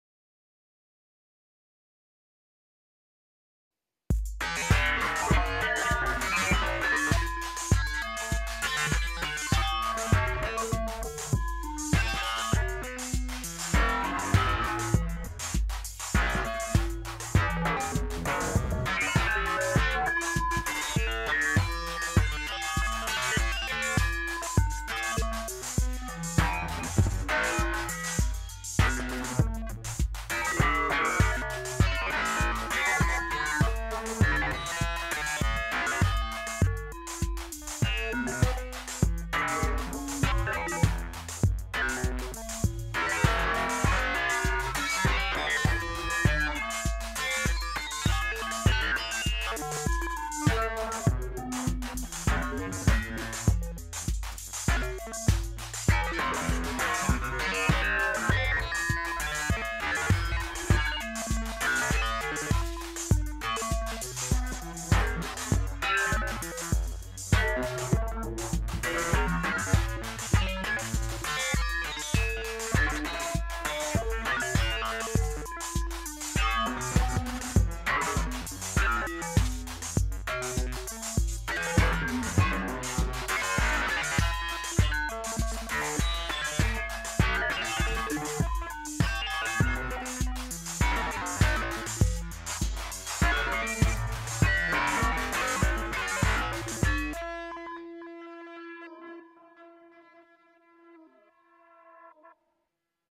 Jamuary2021 | Day 10 | Turing Machine + Basimilus + Desmodus
For the tenth day of Jamuary, let's engineer some noise! The Basimilus and Desmodus make their appearance in this case, so let's put them to work. Once again, the 880 provides backing drums and the clock for the Turing Machine. The Turing Machine, quantized by Disting, drives the Basimilus. The Basimilus then passes through Ripples to take a little of the harsh digital edge off. Veils mixes the sounds and hands them off to Desmodus for some reverb. Random voltages and gates from Marbles, Tides, and Bloom provide additional modulation